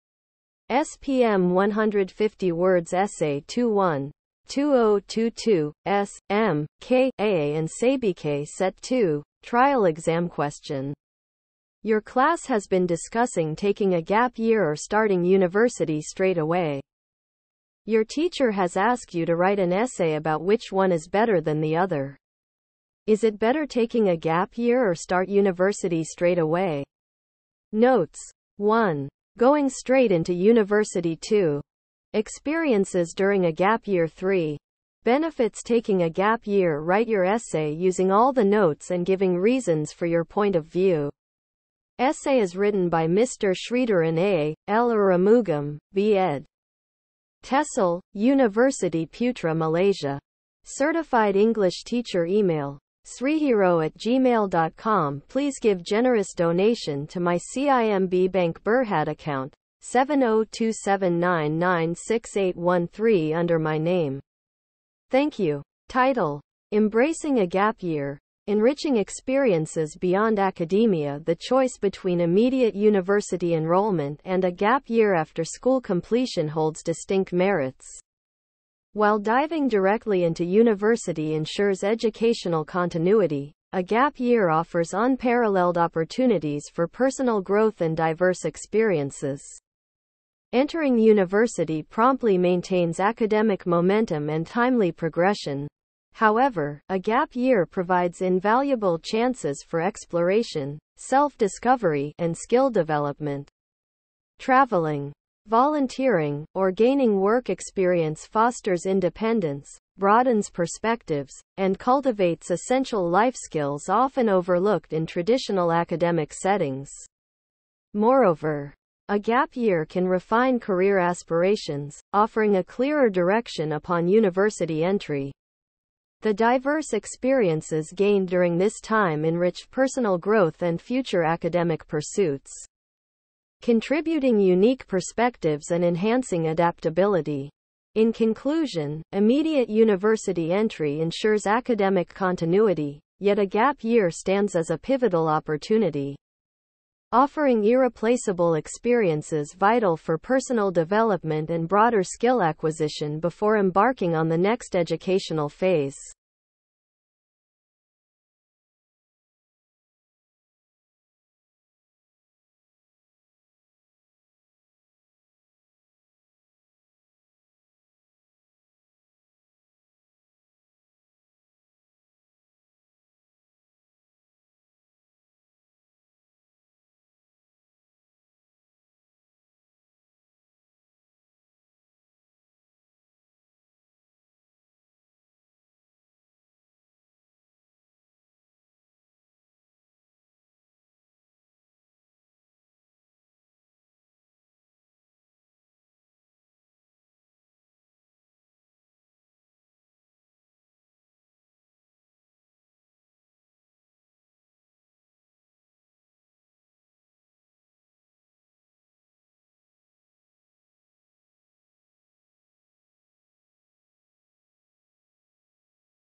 SPM 150 words essay 21: Taking a gap year or start university straight away? 2022 SMKA & SABK Set 2
Use 720p for better image quality.

SPM 150 words essay 21
2022 SMKA & SABK Set 2: Trial exam question.

Your class has been discussing taking a gap year or starting university straight away. Your
teacher has asked you to write an essay about which one is better than the other.

Is it better taking a gap year or start university straight away?

Notes:

1. Going straight into university

2. Experiences during a gap year

3. Benefits taking a gap year

Write your essay using all the notes and giving reasons for your point of view.

Essay is written by Mr Sritharan A/L Arumugam,
B. Ed. (TESL) Universiti Putra Malaysia. Certified English teacher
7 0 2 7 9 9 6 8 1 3 under my name.

Title: Embracing a Gap Year: Enriching Experiences Beyond Academia